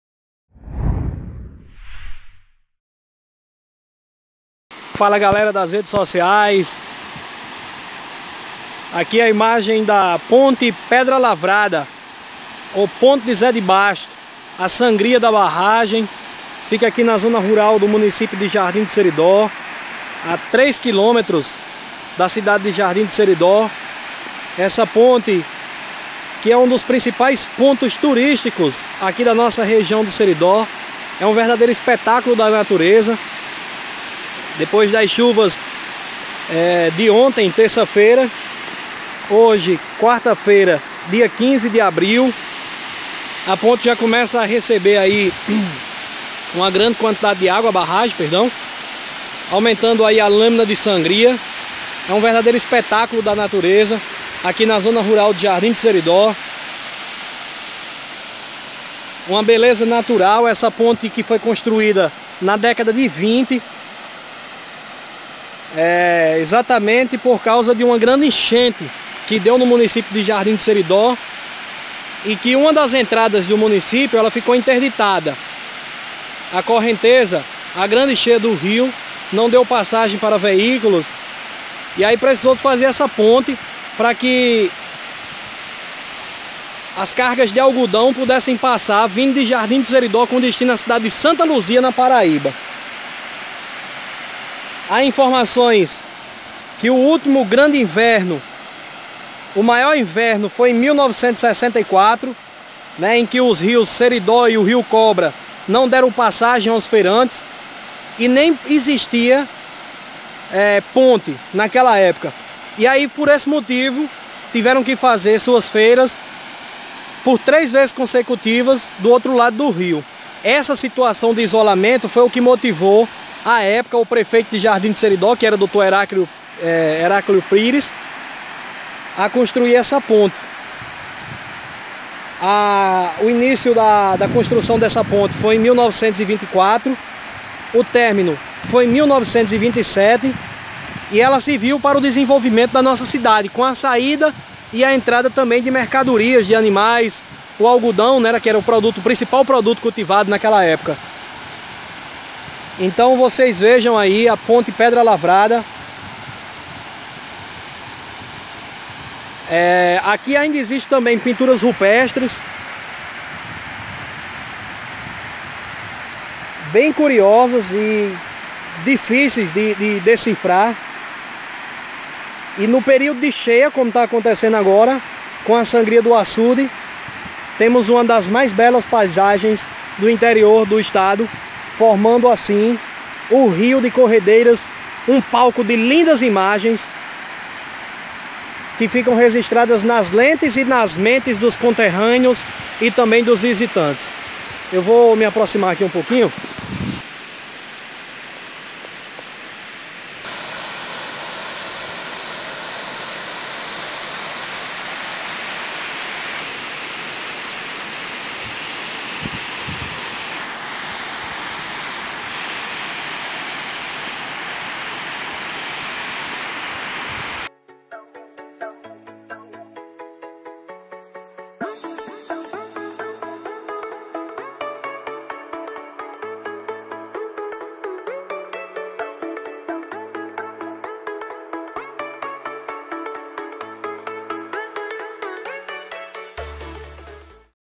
PONTE DA PEDRA LAVRADA, ( Ponte Zé de Bastos ) Jardim do Seridó 15/04/20
Ponte da Fazenda Pedra Lavrada, foi construída sobre o Rio Seridó, zona rural do município de Jardim do Seridó, na gestão de Heráclio Pires, tendo sido inaugurada no ano de 1927, em cerimônia presidida pelo Governador Juvenal Lamartine de Faria.

A construção desta ponte nas imediações da estrada pública que ligava Jardim do Seridó à cidade de Santa Luzia, possibilitou uma via de ligação entre as fronteiras do Rio Grande do Norte com a Paraíba, de modo permanente, evitando tornar a cidade “ilhada”, como aconteceu em 1924, quando as enchentes dos dois rios que cortam a cidade impediram a entrada e saída das pessoas.

vídeo feito pelo amigo

SE VOCÊ GOSTA DE FAZER VÍDEO E QEIRA QUE POSTE PODE ENVIAR PELO MEU ZAP: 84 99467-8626

TV TRÊS RIOS WEB CRUZETA - RN